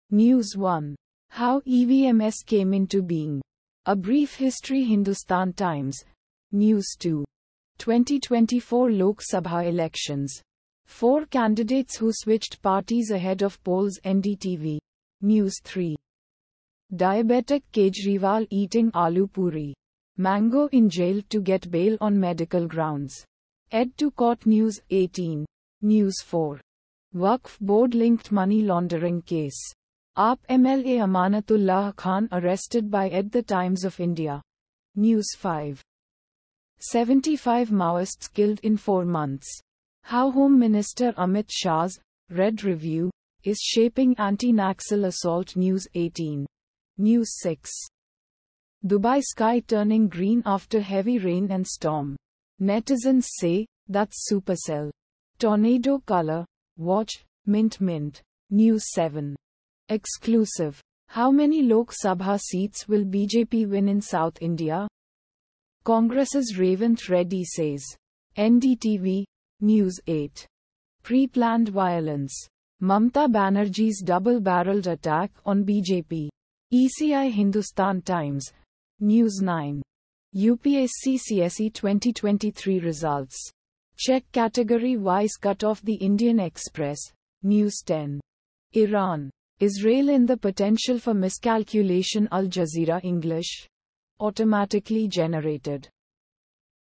News Headlines (India)- 20/04/2024,03:44 AM GMT+5:30
News 1:How EVMs came into being: A brief history - Hindustan Times
News 2:2024 Lok Sabha Elections: 4 Candidates Who Switched Parties Ahead Of Polls - NDTV
News 3:Diabetic Kejriwal Eating Aloo Puri, Mango In Jail To Get Bail On Medical Grounds: ED To Court - News18
News 4:Waqf Board-linked money laundering case: AAP MLA Amanatullah Khan arrested by ED - The Times of India
News 5:75 Maoists Killed In 4 Months: How Home Minister Amit Shah's 'Red Review' Is Shaping Anti-Naxal Assault - News18
News 6:Dubai sky turning green after heavy rain and storm, netizens say 'That’s supercell, tornado color | Watch | Mint - Mint
News 7:Exclusive: How Many Lok Sabha Seats Will BJP Win In South India? Congress's Revanth Reddy Says... - NDTV
News 8:‘Pre-planned violence’: Mamata Banerjee’s double-barrelled attack on BJP, ECI - Hindustan Times
News 9:UPSC CSE 2023 Results: Check category-wise cut-off - The Indian Express
News 10:Iran, Israel and the potential for miscalculation - Al Jazeera English

Automatically Generated...